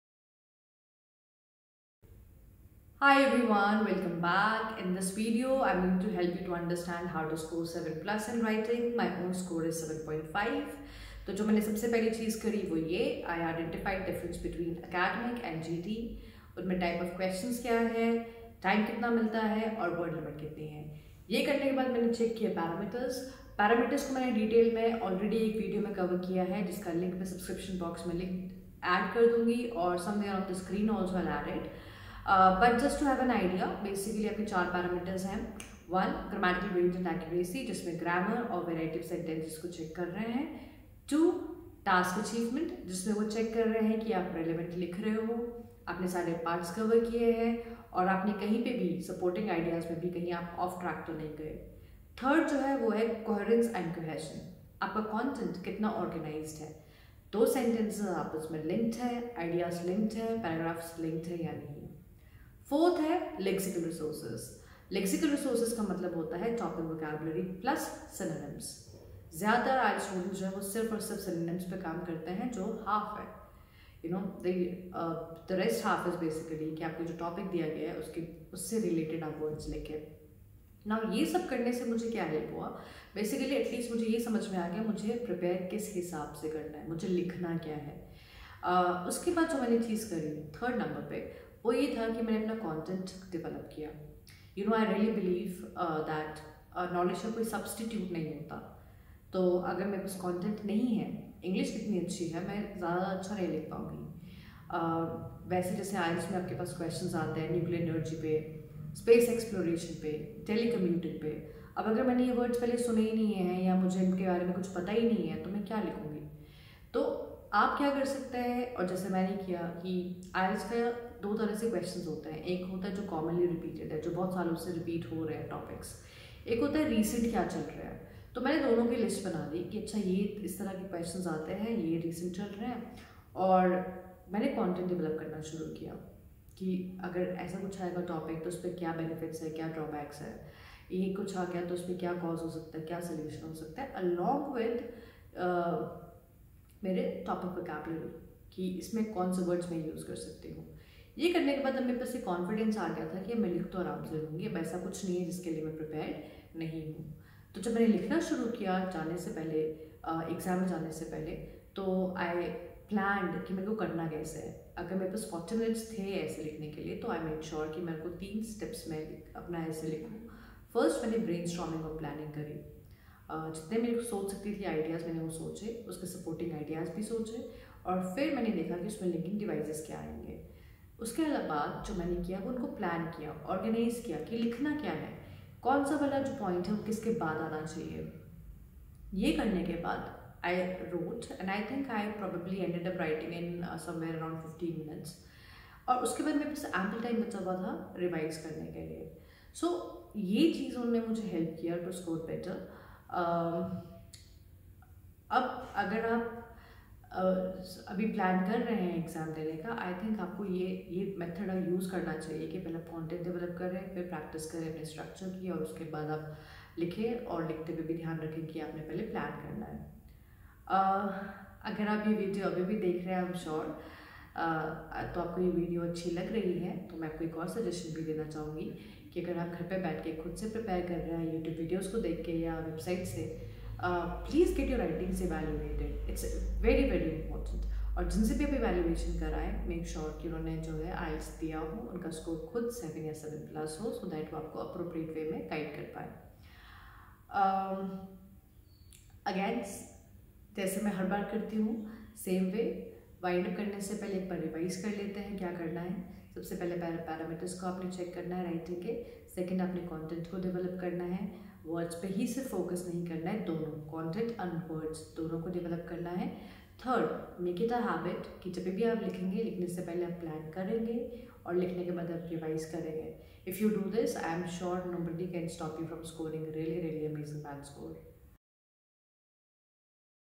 HOW I SCORED 7.5 BANDS IN IELTS WRITING || STUDY PLAN
Writing is considered to be the most challenging module. however, if attempt to only write keeping the parameter in mind, it can be the easiest as well.
Follow simple steps:
1. Understand the parameters.
2 . Read extensively to have ideas.
3. Plan before writing and revise after that.